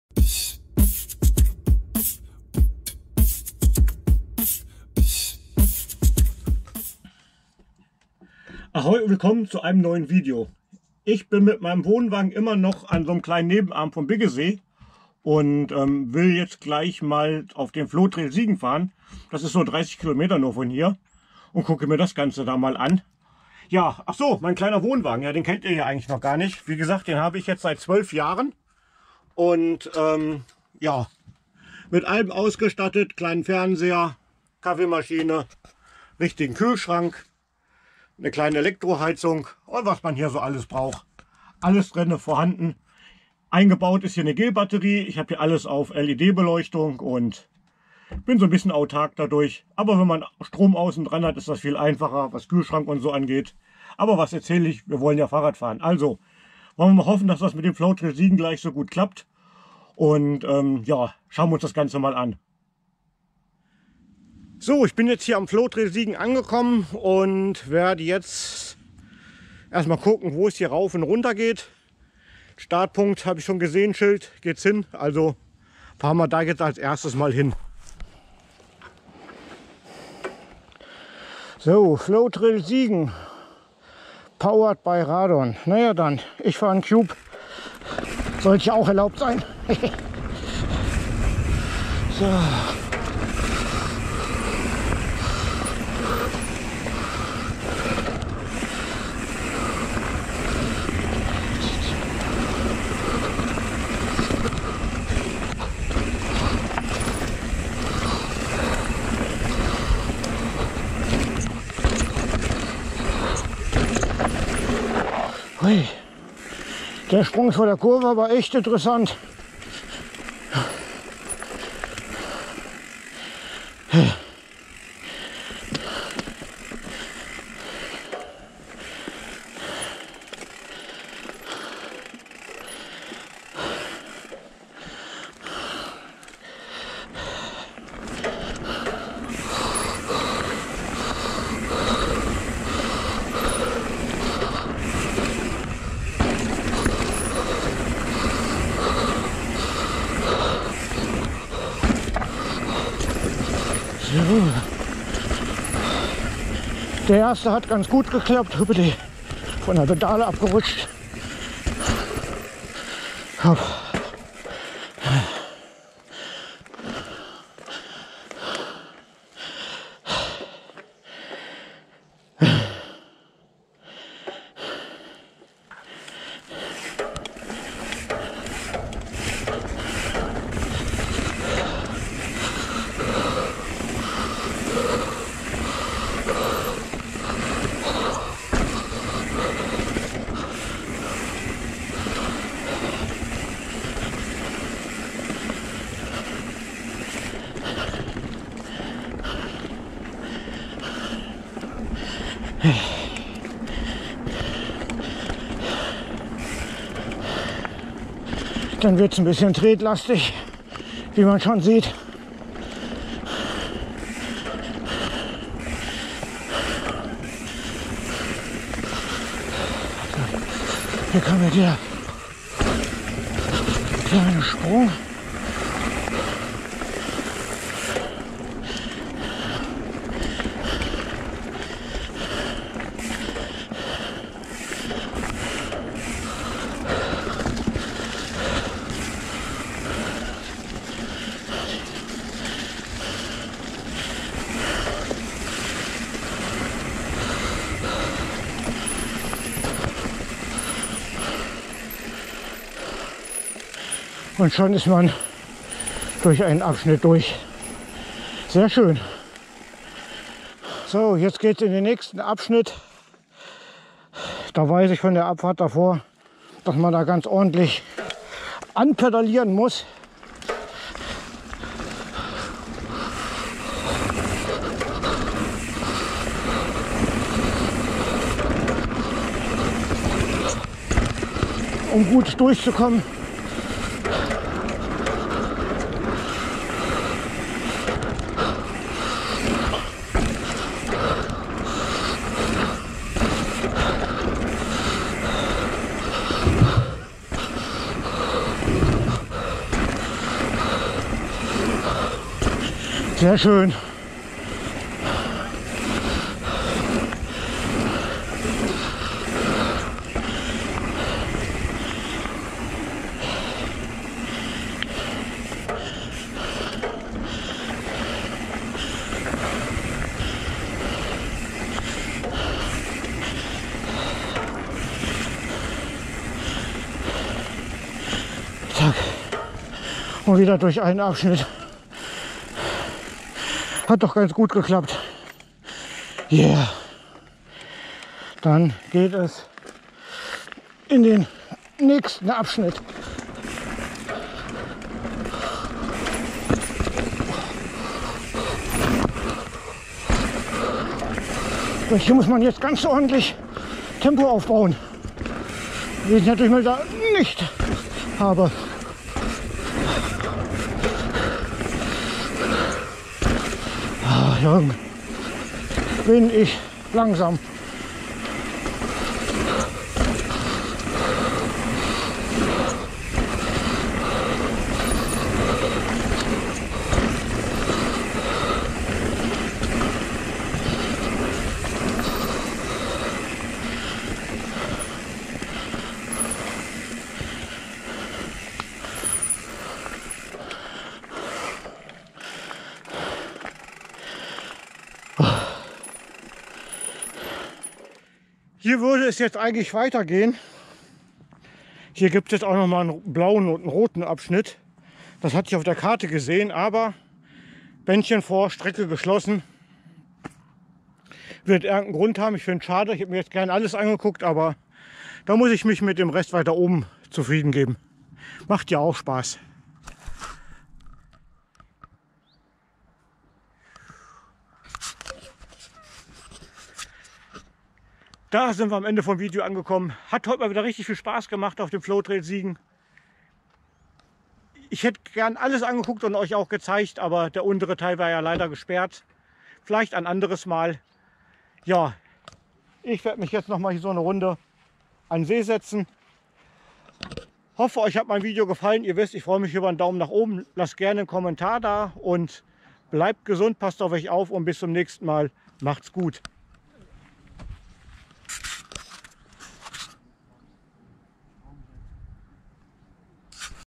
Flowtrail Siegen / Cube Stereo 170 / Super fürs Enduro
Das war mein erster Besuch auf dem Flowtrail Siegen. Gefällt mir sehr gut, und da ja leider die untere Hälfte gesperrt war, werde ich ich noch mal wiederkommen.

Video und Bild Bearbeitung:
-Canva